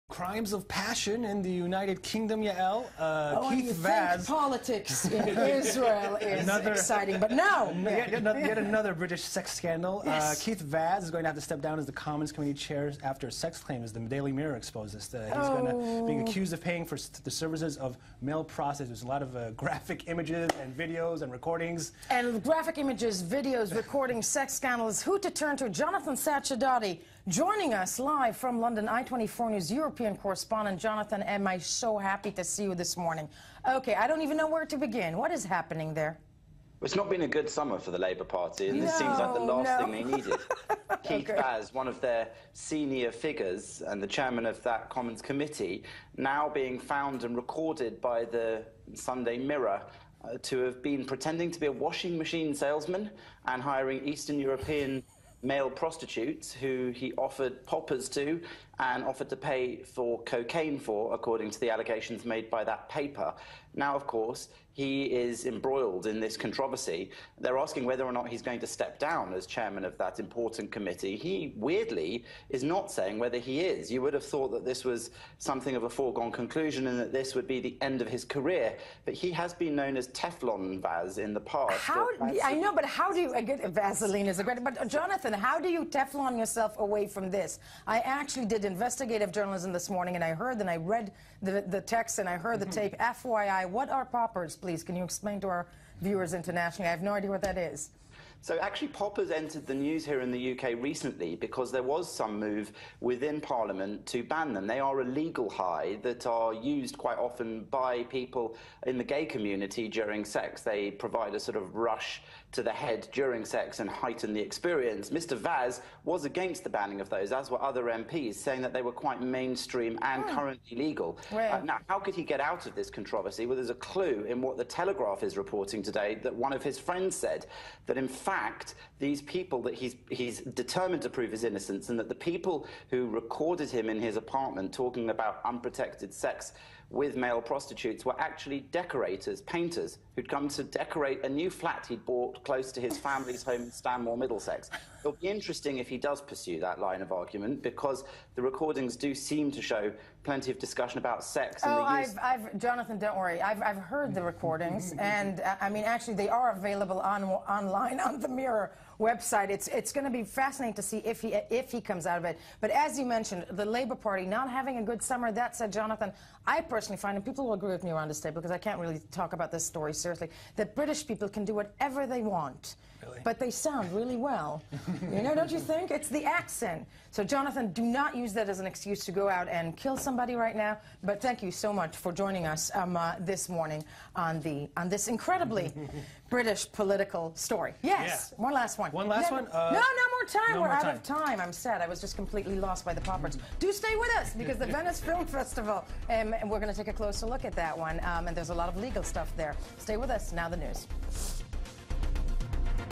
Jonathan Sacerdoti reports on allegations Keith Vaz paid male prostitute for sex and drugs
i24news UK and Europe correspondent Jonathan Sacerdoti reports on the Keith Vaz male prostitute and drugs allegations.